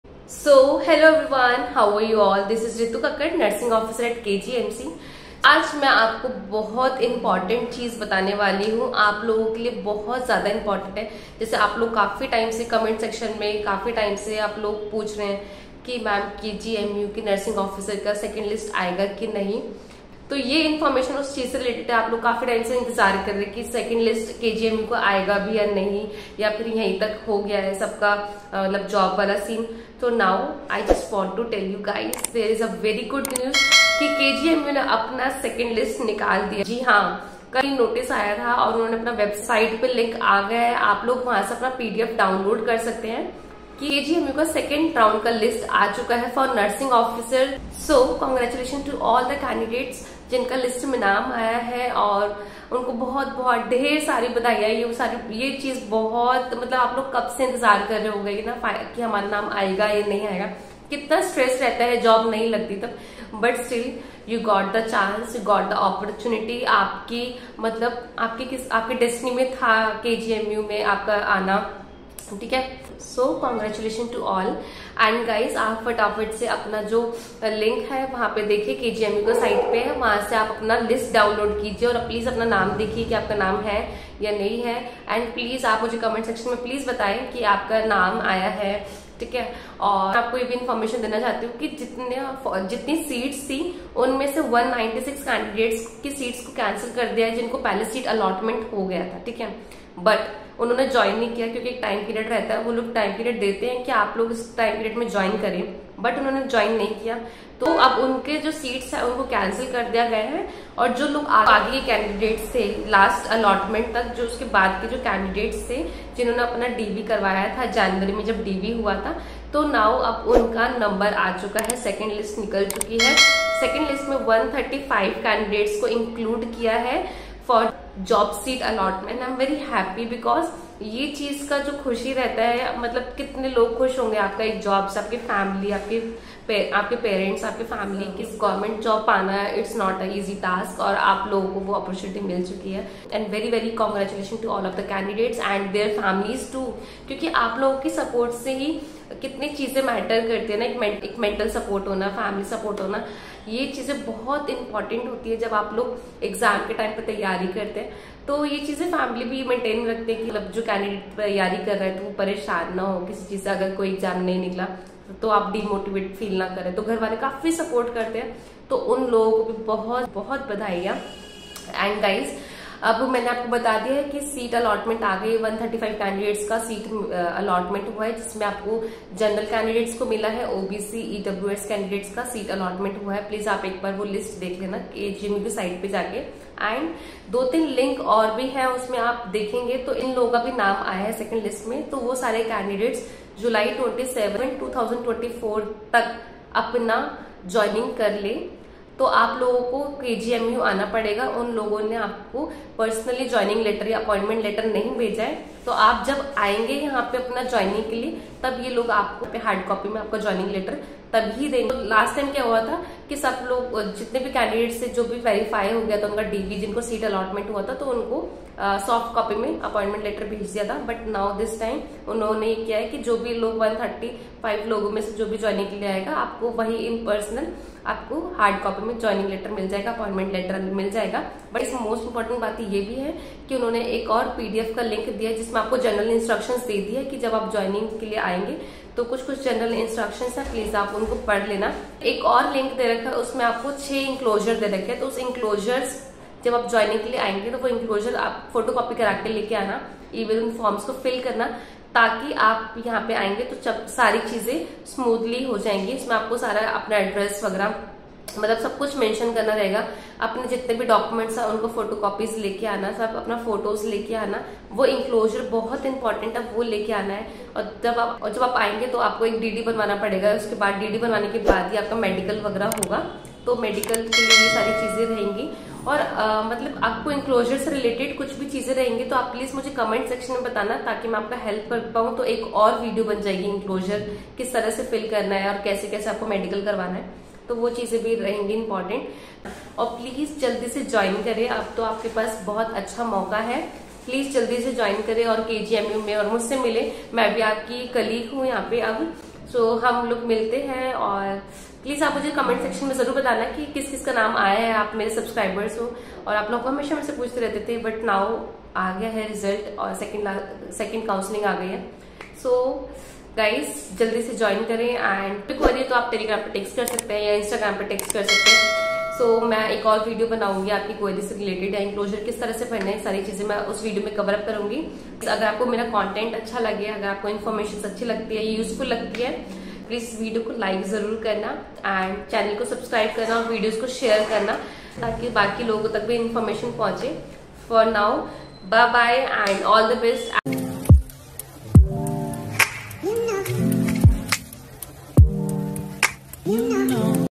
Finally 2nd list released KGMU Nursing Officer Exam 2024
2nd list released  KGMU Nursing Officer Exam

Msc nursing ke sath Job kr rhi hu?!! Konse hospital mein job lagi hai Msc k sath job kaise kar sakte

From Delhi Punjabi Bagh (India)